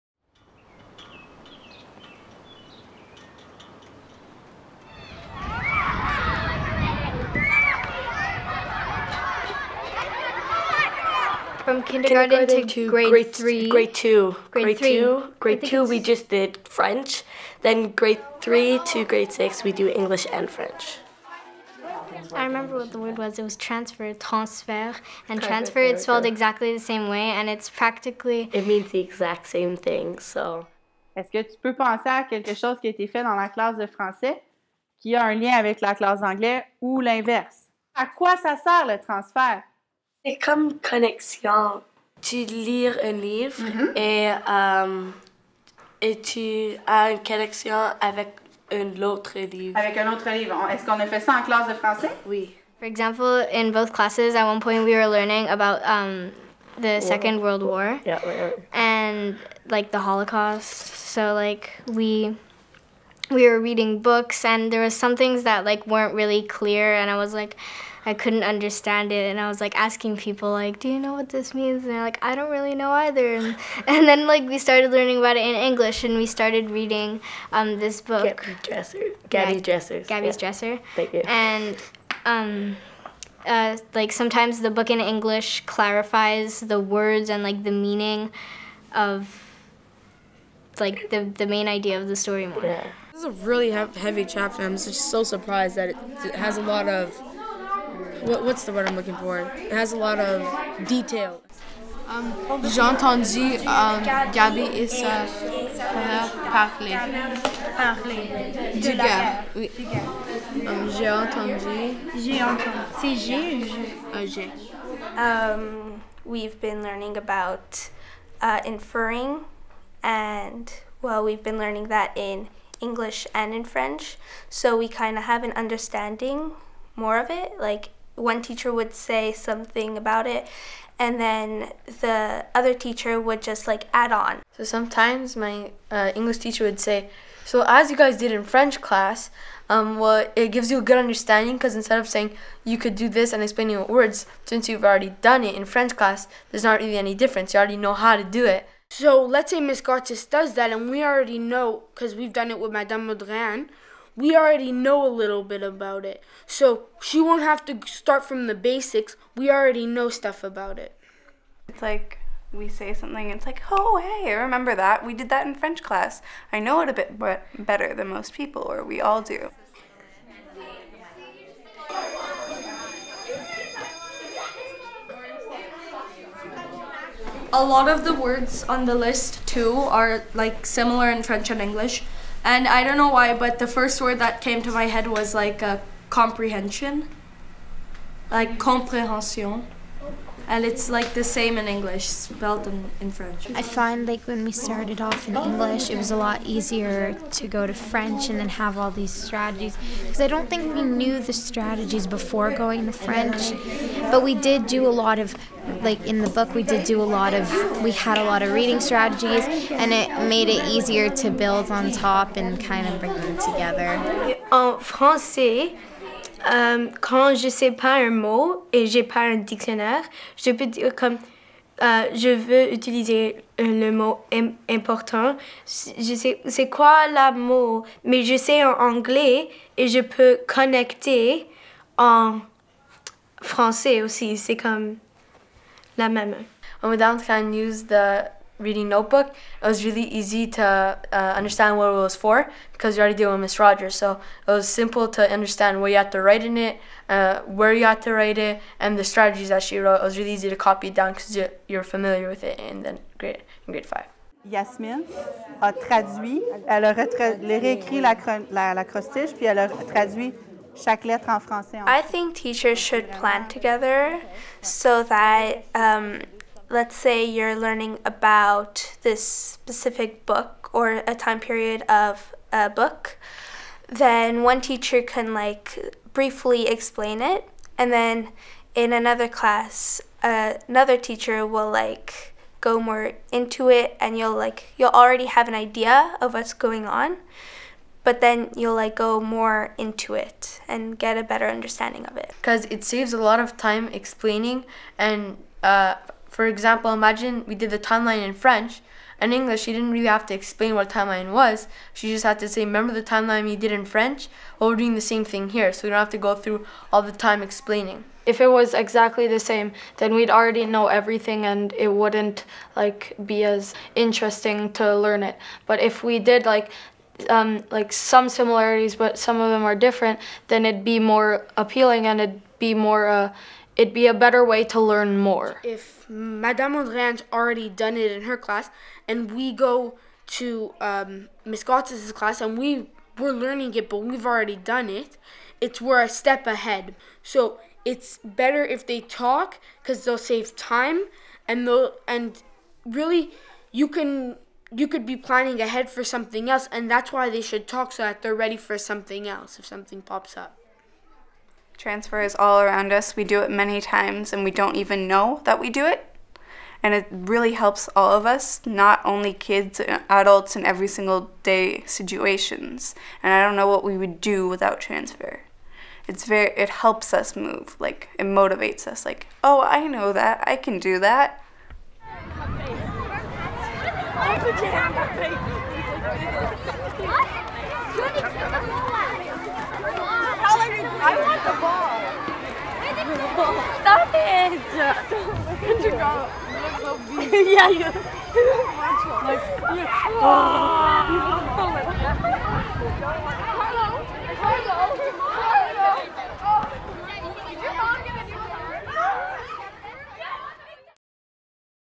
La parole est aux élèves
Vidéo démontrant des élèves de 6e année (primaire), du programme d'immersion française, qui partagent leurs réflexions sur le transfert des apprentissages entre les langues.

Cette vidéo a été tournée à l’école Merton, de la Commission scolaire English-Montréal. On y voit des élèves de 6e année, du programme d’immersion. Ces élèves ont participé à un projet où leurs enseignantes, en English Language Arts et en français, langue seconde, ont mis en œuvre des pratiques d’enseignement favorisant le transfert des apprentissages langagiers. Au terme du projet, les élèves ont partagé leurs réflexions sur le transfert des apprentissages langagiers.